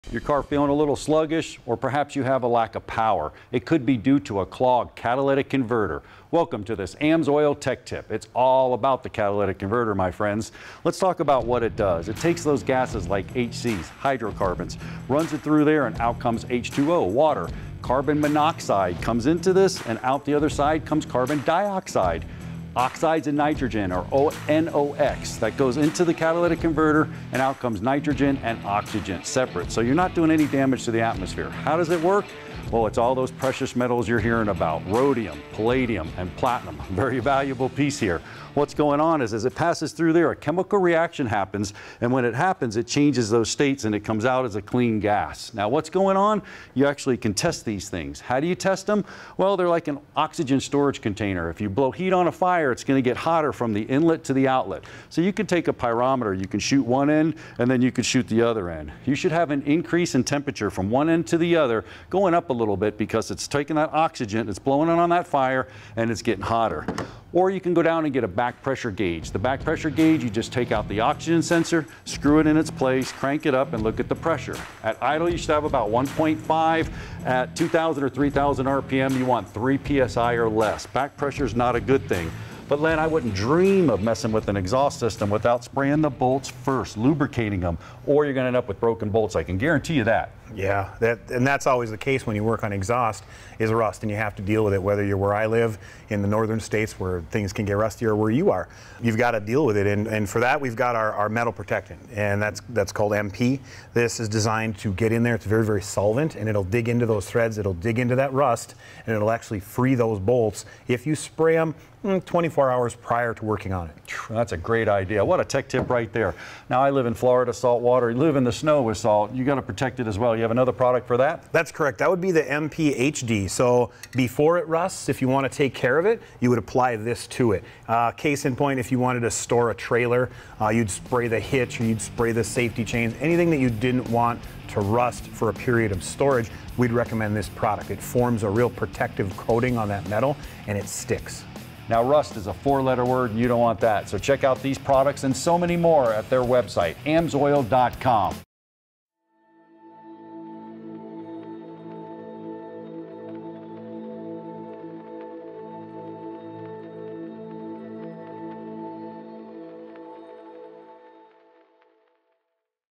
How to Test Your Catalytic Converter
In this week’s episode of Tech Tips, we’re talking about catalytic converters.

Find out how to test your exhaust system and protect it using AMSOIL MP Metal Protector.